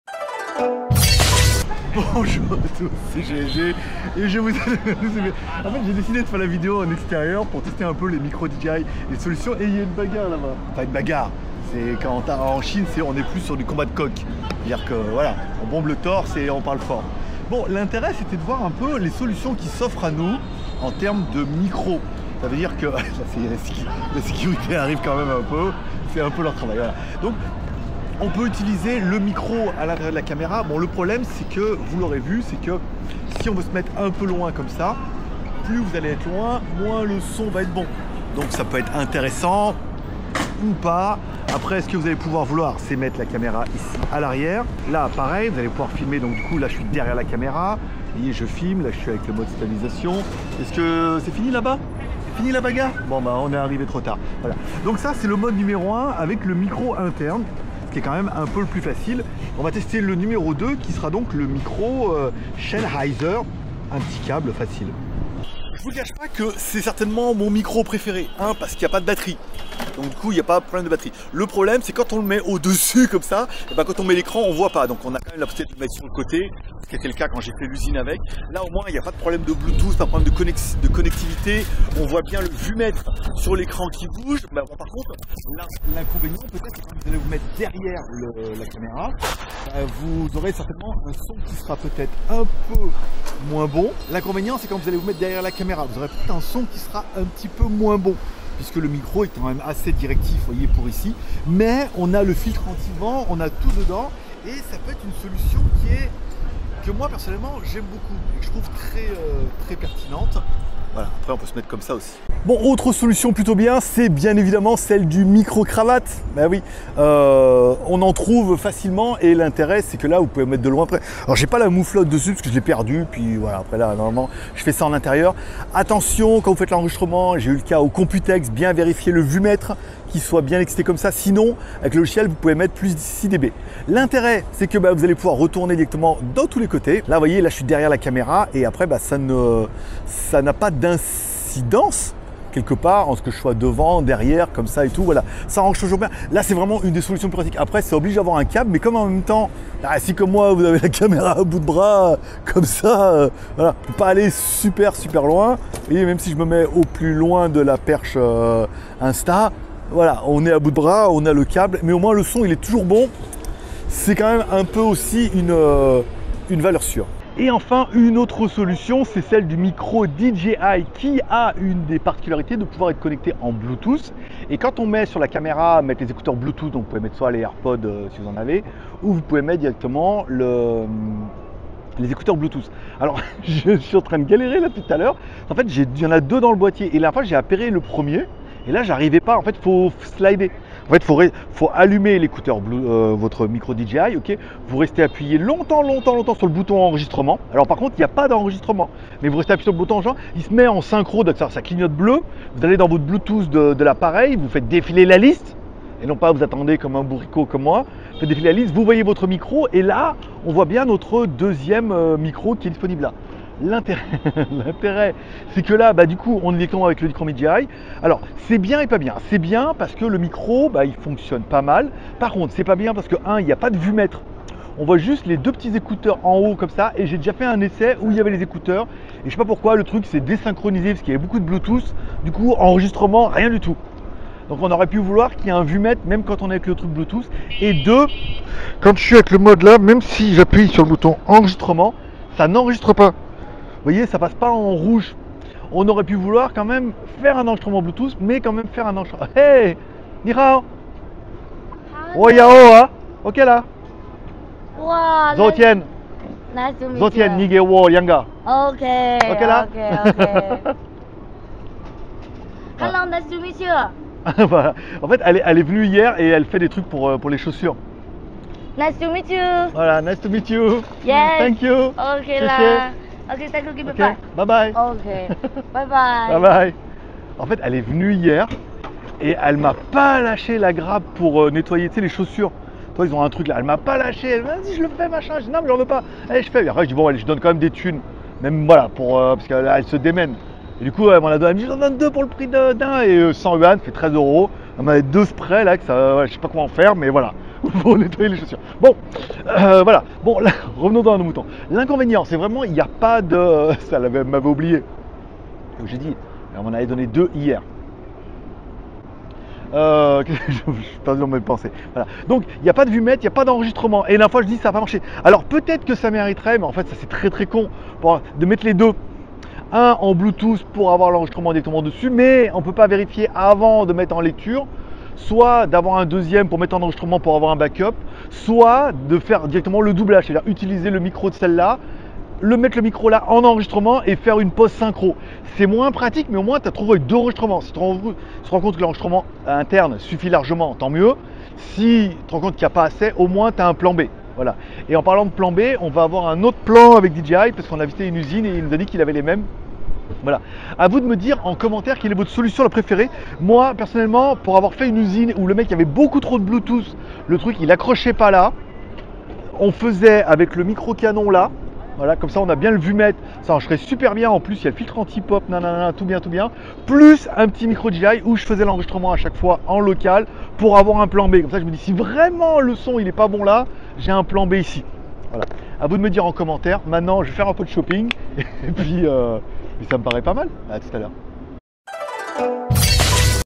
Test de 4 Micros pour Insta360 Ace Pro : Qui S'en Sort le Mieux ?
Dans cette vidéo, je teste 4 micros pour la caméra Insta360 Ace Pro : le micro interne, un micro cravate, un micro canon, et le micro Bluetooth DJI. Lequel offre la meilleure qualité sonore pour vos vidéos ? Découvrez-le dans ce test complet !
Acheter la caméra Insta360 Ace Pro et
Acheter le Micro Cravate
Acheter le Micro Canon MKE 200
Acheter le Micro Bluetooth DJI

By GLG